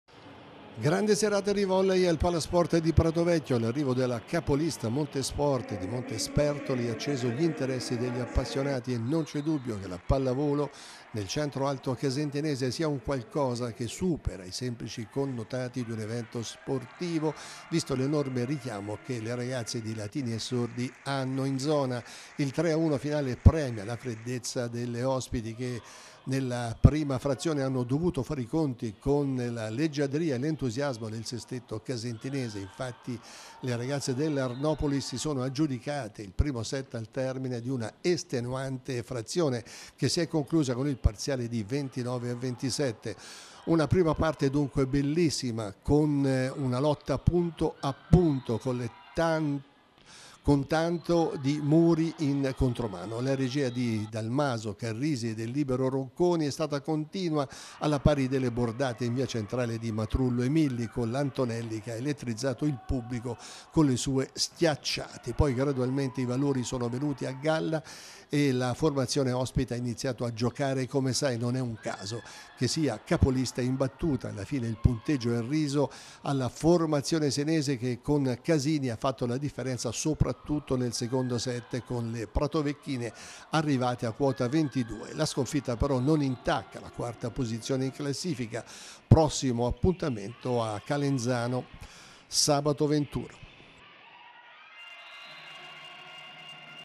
Spettacolo a Pratovecchio passa la capolista 1-3
La capolista Montesport conferma a Pratovecchio il motivo perchè è al comando della classifica del girone H della B2 femminile.